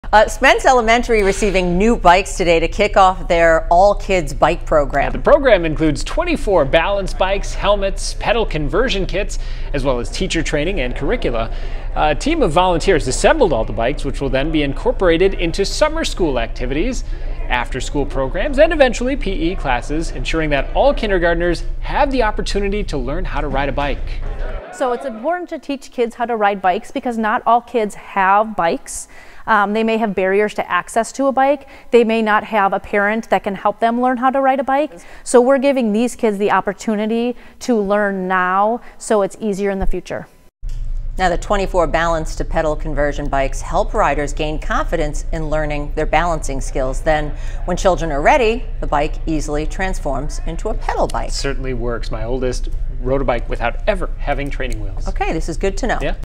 Local elementary school wheels in new learn-to-ride curriculum
Spence Elementary School received new bikes to wheel in their All Kids Bike program.